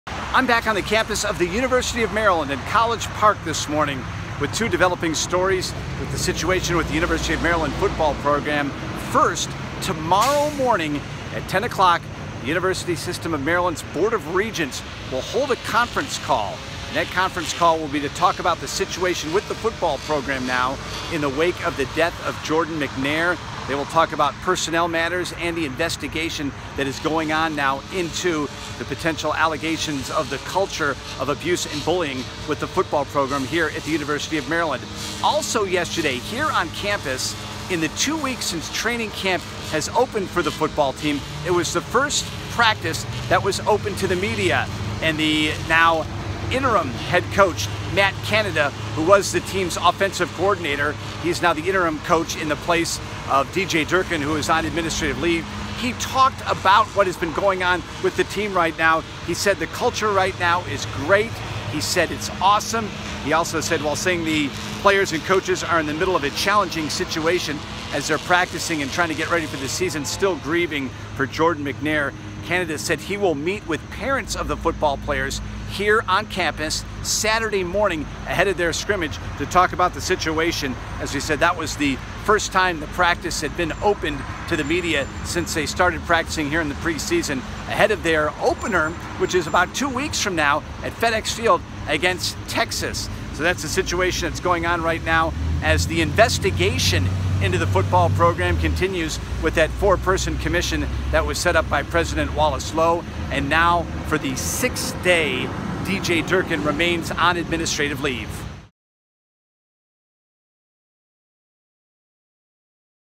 University System To Hold Special Meeting
The Terrapins football team got back to practice Wednesday, a day after the University of Maryland's president and athletic director acknowledged it is the university's fault for the death of Jordan McNair.

The focus at practice shifted to a safety-first environment with cooling tents and cold water tubs situated around the practice field.

"Our practices have been extremely crisp. The focus of our players health and safety is number one," said interim head coach Matt Canada.